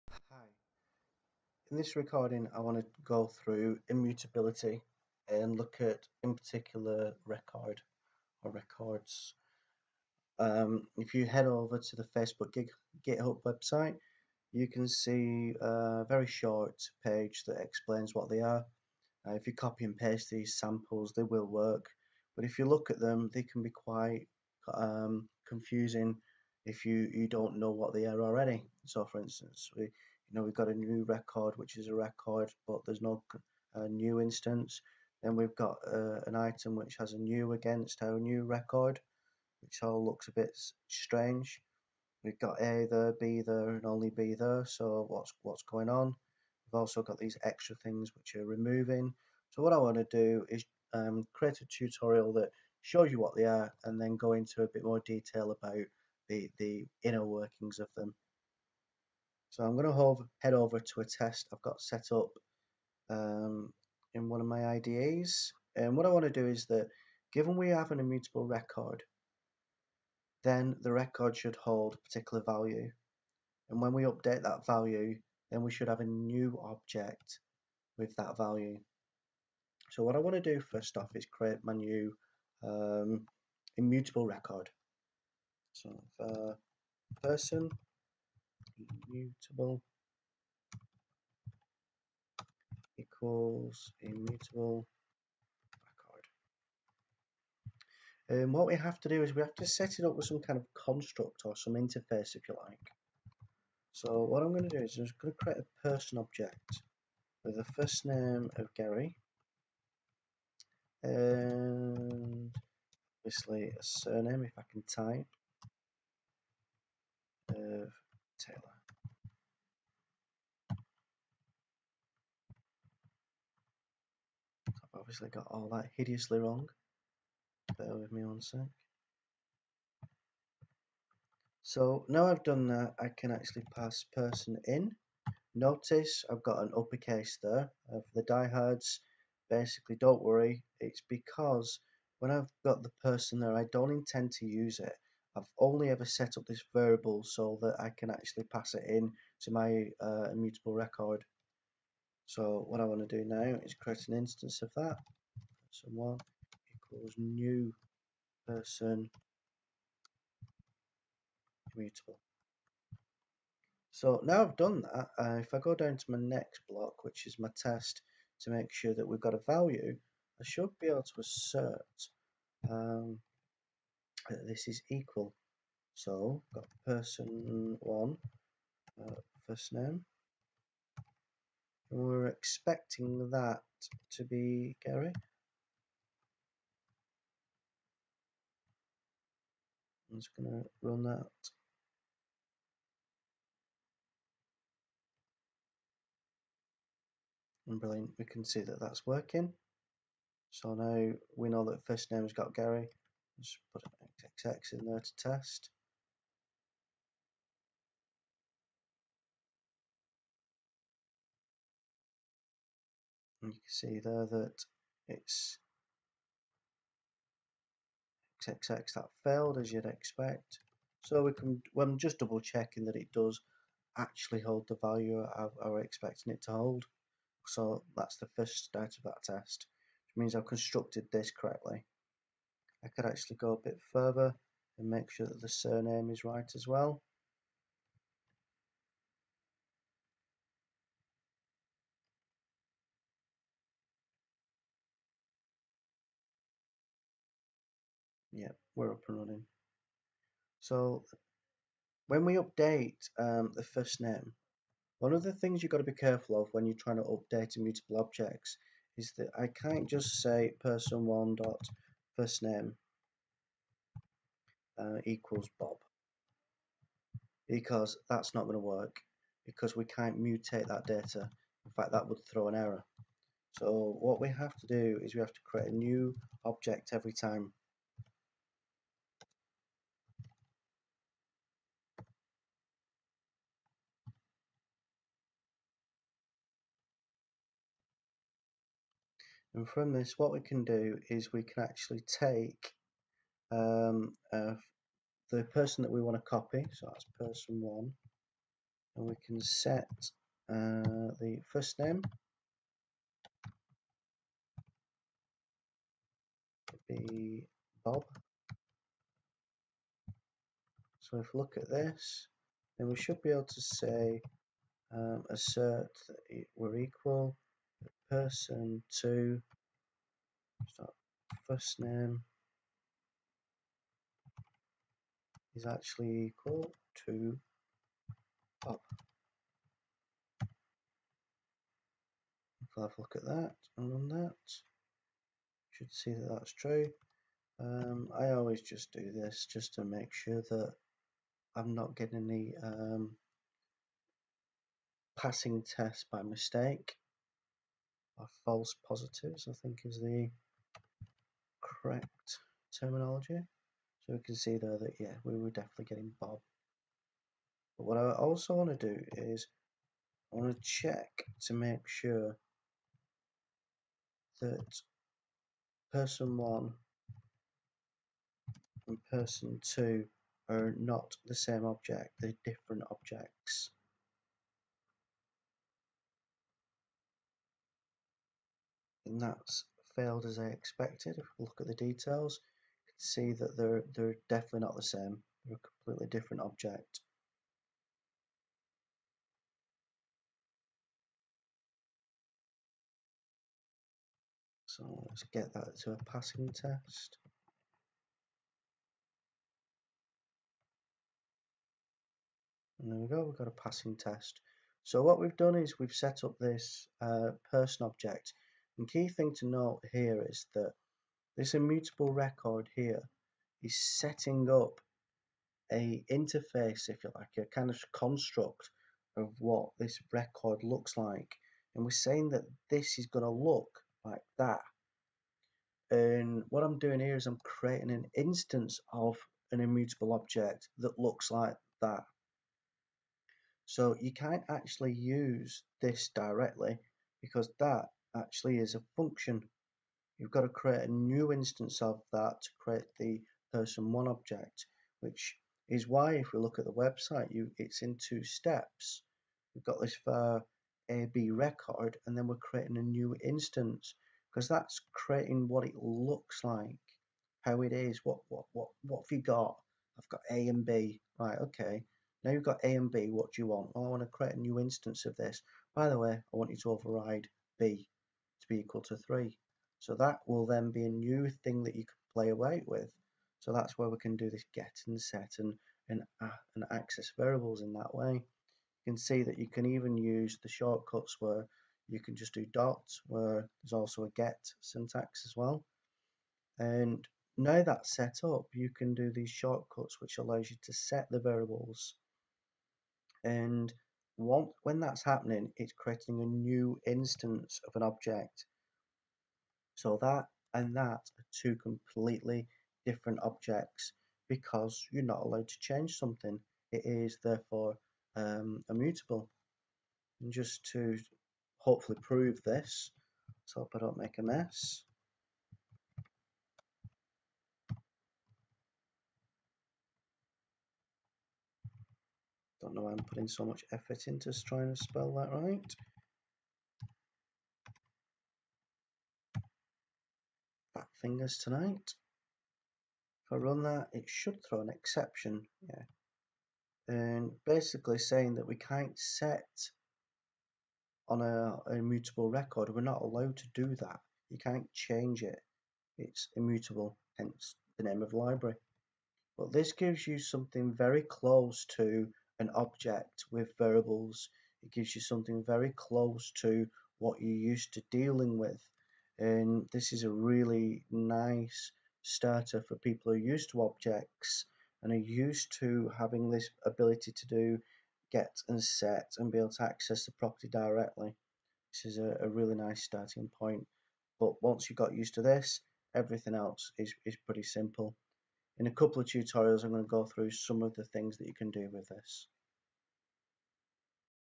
Intro to Immutable Records
In this vid I try to explain how to use Immutable Records. This includes setting a record up and updating values. The purpose of this vid is to give a very simple overview of what a Record is and why you might use it.